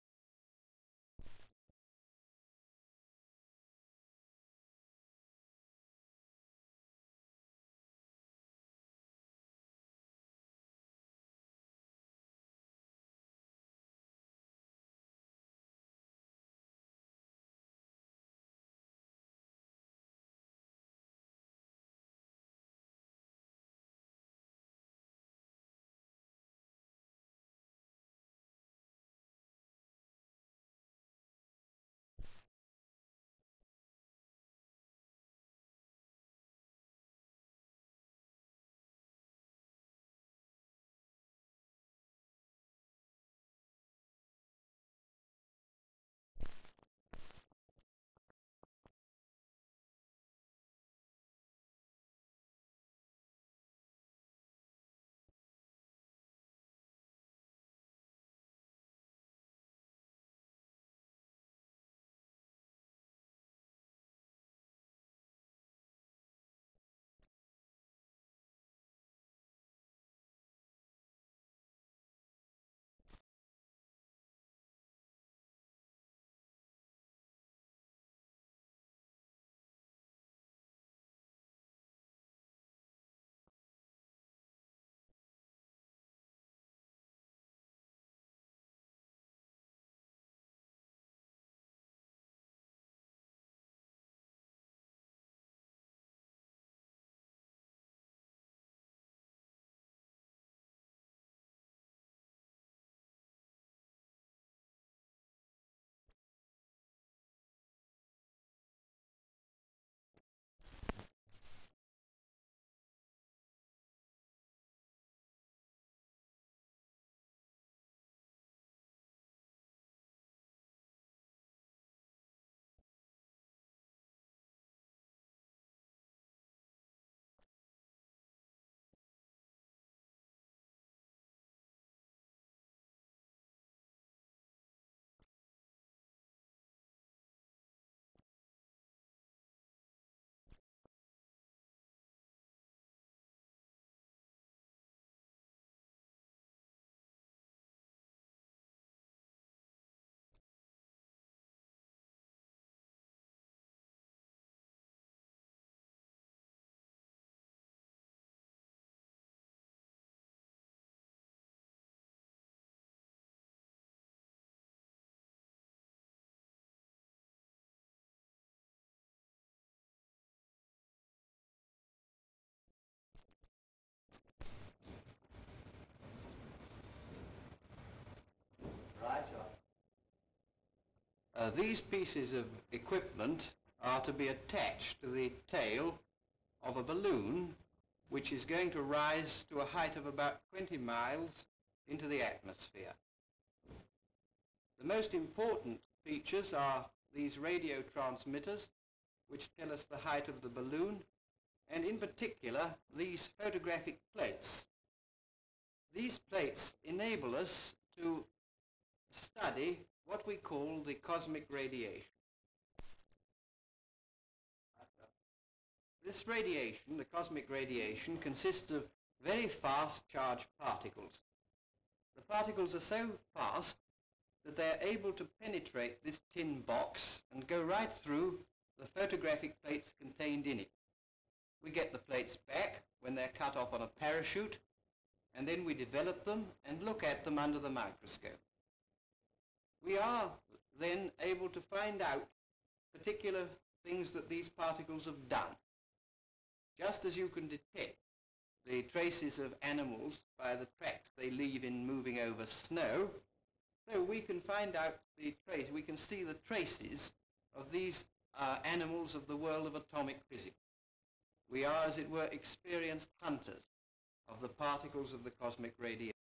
Professor Powell's Stratosphere Balloon (1950)
Professor Powell's stratospheric balloon. Bristol.

CU unrolling long sheet of plastic onto bench. MS smoothing out plastic sheet. Various shots marking out panels on sheet. MS looking through sheet as man marks panels. CU girl cutting panels in plastic sheet. Various shots girls sealing the panels with an electric iron.  CU tin box placed onto panel. CU clock. Various shots assembling the clock apparatus. LS girls working sewing machines. CU girls examining panels through microscopes. CU apparatus to show speed of ascent.

MS Professor Powell speaking:- (natural sound) "These pieces of equipment are to be attached to the tail of a balloon, which is going to rise to a height of about 20 miles into the atmosphere. The most important features are these radio transmitters which tell us the height of the balloon, and particularly these photographic plates, these plates enable us to study, what we call cosmic radiation. This radiation, the cosmic radiation, consists of very fast charged particles the particles are so fast that there are able to penetrate this tin box and go right to the photographic plates contained in it. We get the plates back when they are cut off of in a parachute and then we develop them and look at them under a microscope, we are then able to find out particular things that these particles have done just as you can detect the traces of animals by the tracks they leave in moving over snow so we can find out, we can see the traces of these animals of the world of atomic physics. We are, as it were, experienced hunters of the particles of the cosmic radiation".
 FILM ID:2577.06